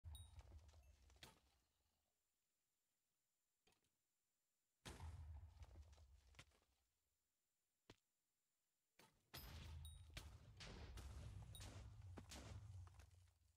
Even the dungeon mobs are cheating now.
Cheating has become so rampant that even the dungeon mobs are vclipping.